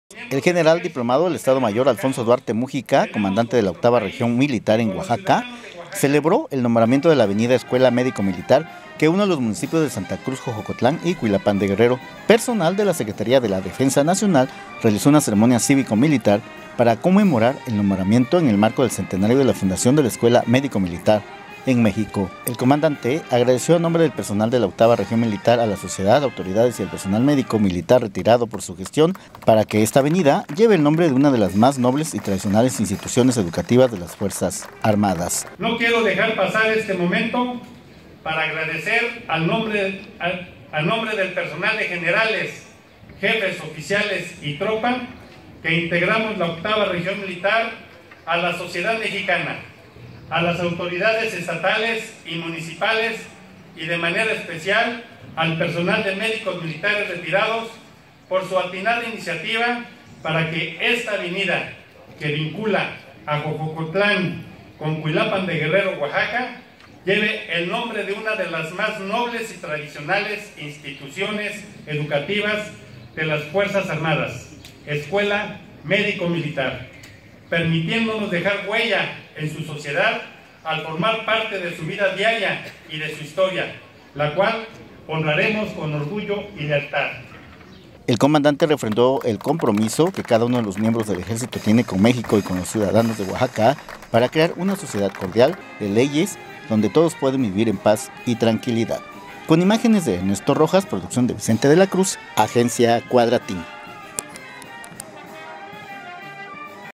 Celebra Sedena nombramiento de avenida Escuela Médico Militar
OAXACA, Oax., 1 de julio de 2017.- El general diplomado de Estado Mayor, Alfonso Duarte Múgica, comandante de la Octava Región Militar de Oaxaca, celebró el nombramiento de la avenida Escuela Médico Militar, que une a los municipios de Santa Cruz Xoxocotlán y Cuilápam de Guerrero.
Personal de la Secretaría de la Defensa Nacional (Sedena) realizó una ceremonia cívico-militar para conmemorar el nombramiento, en el marco del Centenario de la Fundación de la Escuela Médico Militar en México.
Como parte de la celebración, la banda del Ejército tocó música popular y se sembró un árbol de pochote, con el que se develó una placa.
El comandante agradeció a nombre del personal de la Octava Región Militar a la sociedad, autoridades y al personal médico militar retirado por su gestión para que esta avenida lleve el nombre de “una de las más nobles y tradicionales instituciones educativas de las fuerzas armadas”.
El comandante refrendó el compromiso que cada uno de los miembros del Ejército tiene con México y con los ciudadanos de Oaxaca para crear una sociedad cordial, de leyes, donde todos puedan vivir en paz y tranquilidad.